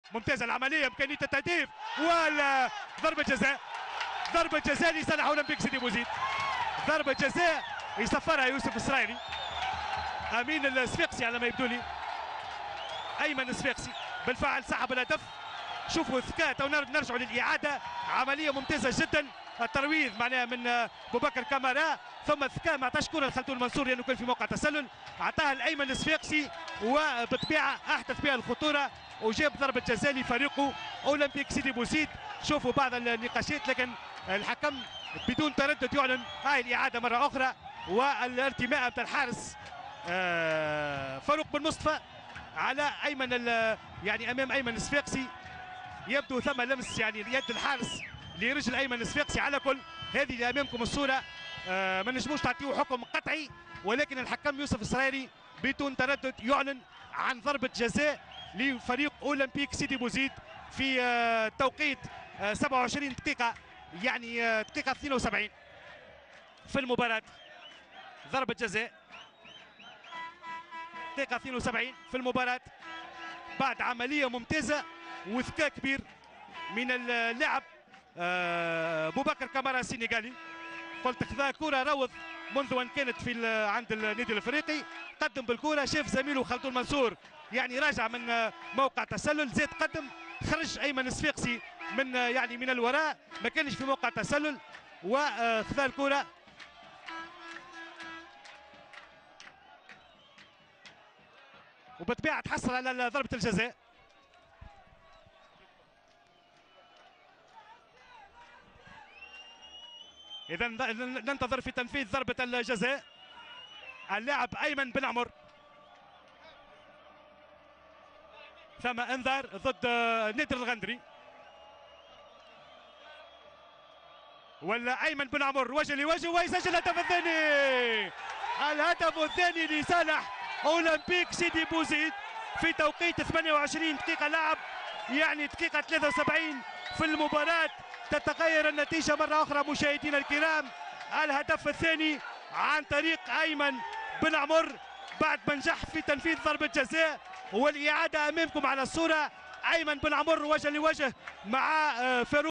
OSBTv OSB 2-0 CA - But 2: Aymen Ben Amor
OSB 2-0 CA
J2 saison 2015/2016
Buts:
1- Aymen Sfaxi 15"
2- Aymen Ben Amor 72"